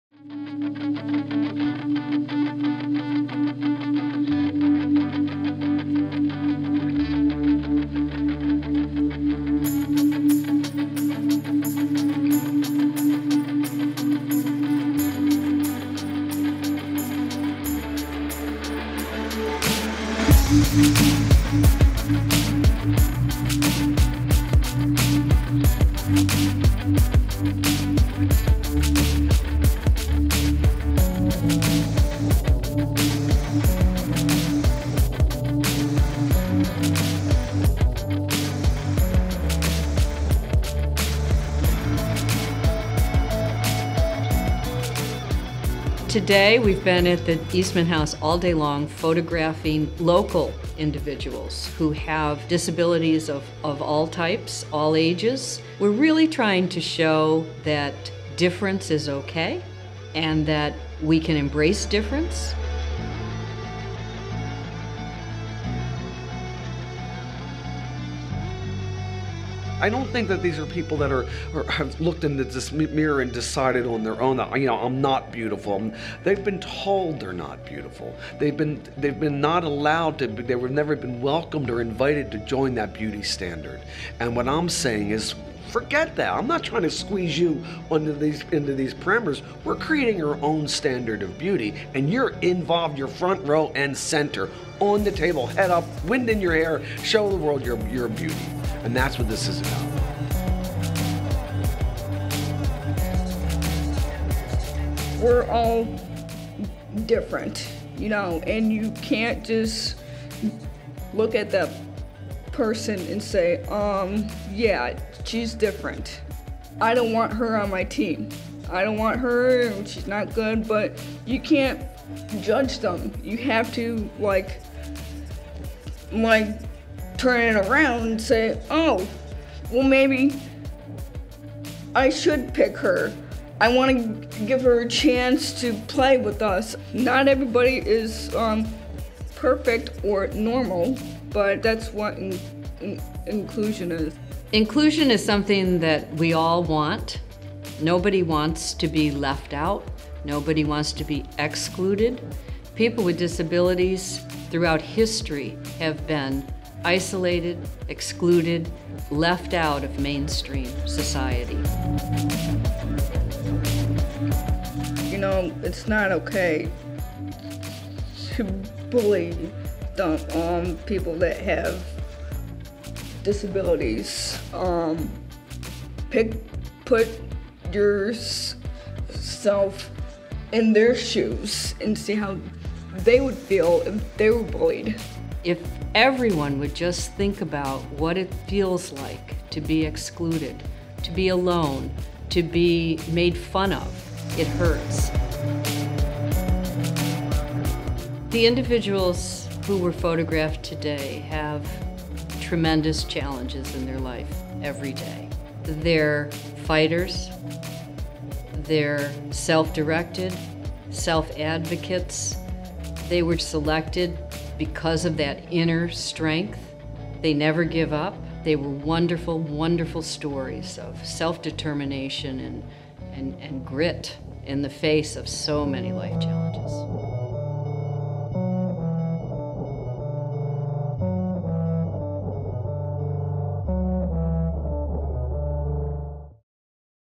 Homework Hotline Move to Include: Inclusion
In this Move to Include video from Homework Hotline, we will meet Cori, Rick and Ann to talk about a project called Positive Exposure and learn about inclusion.  Rick is a photographer who highlights the beauty of people with genetic differences. We hear about what it’s like to be excluded and why inclusion is so important.